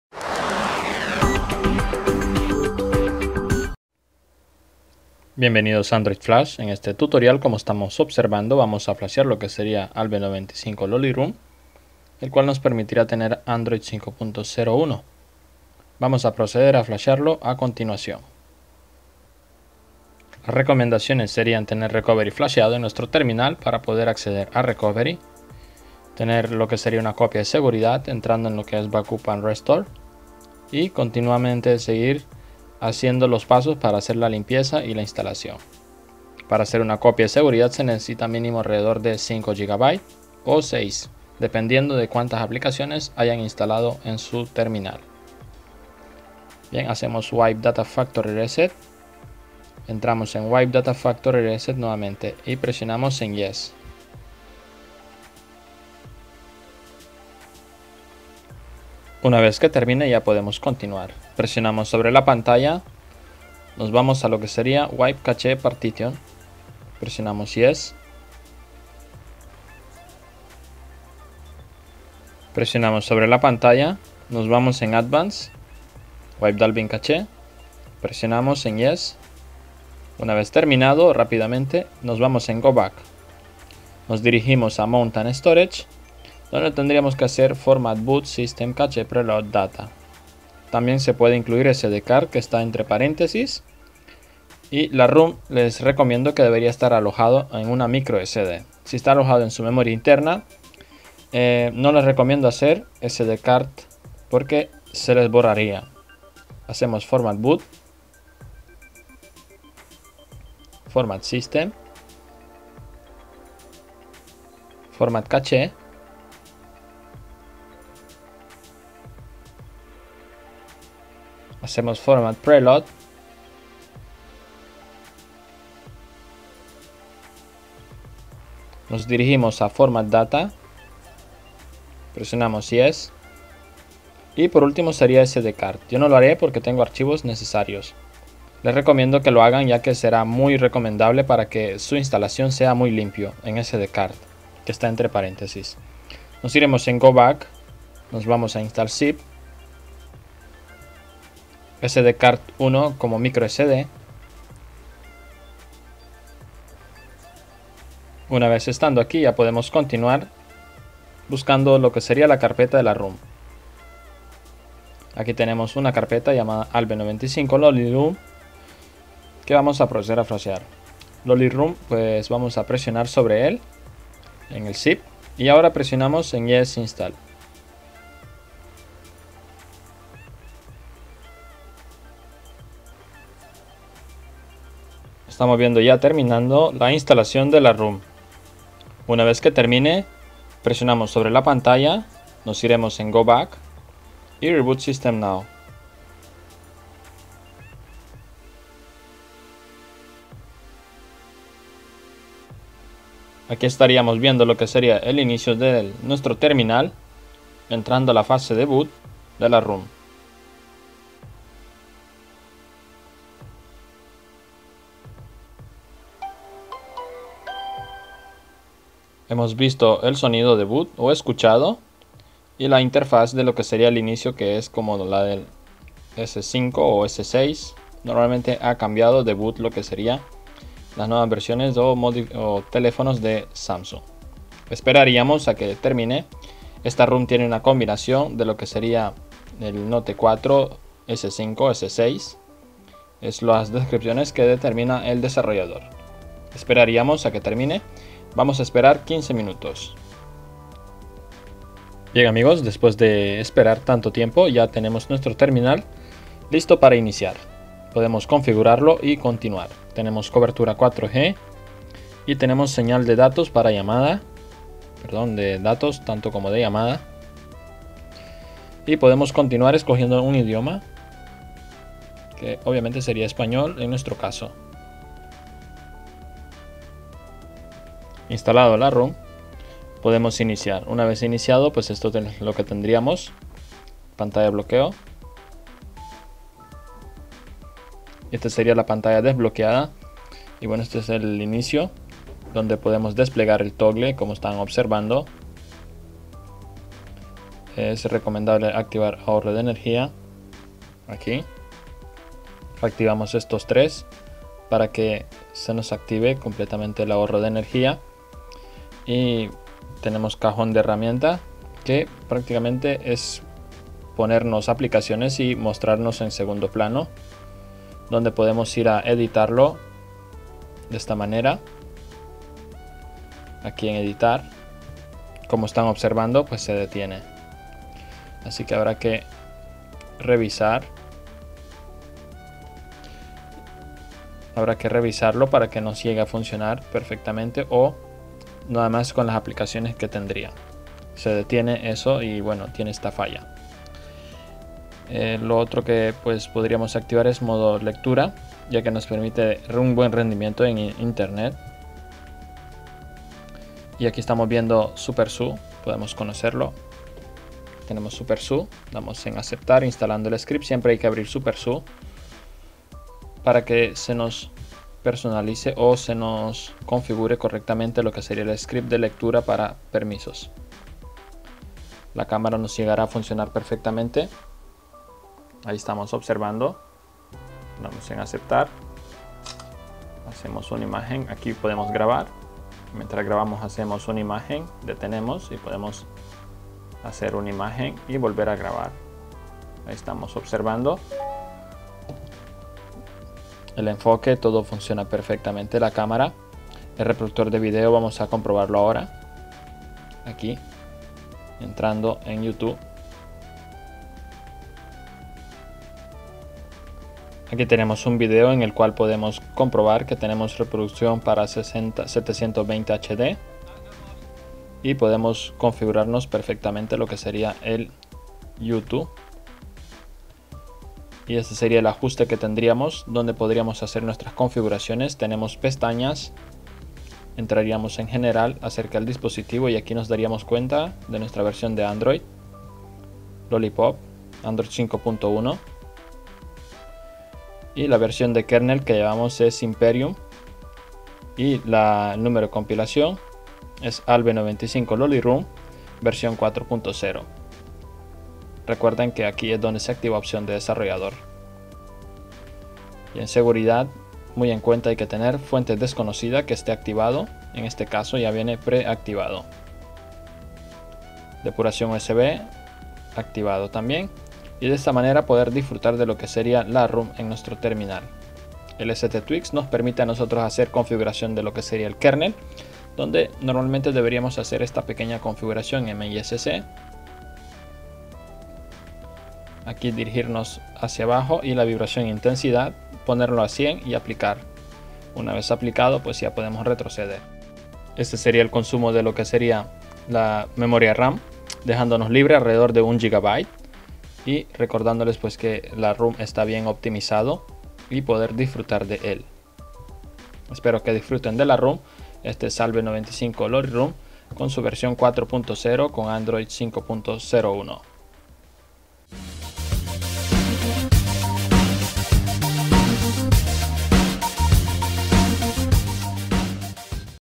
ALBE95 LOLLIROM V4.0 STOCK 5.0.1 PARA S4
HOLA AMIGOS.

AQUI LES DEJO TODA LA INFORMACIÓN NECESARIA PARA PODR PERSONALIZAR EL TELEFONO.

FORO PARA AYUDA Y SOPORTE EN XDA INGLES

EN ESTA SECCIÓN PODRÁN CONOCER COMO HACER UNA COPIA DE SEGURIDAD PARA PODER FLASHEAR

EN ESTA SECCIÓN ENCUENTRAN EL ENLACE A LA ROM

!GRACIAS^^*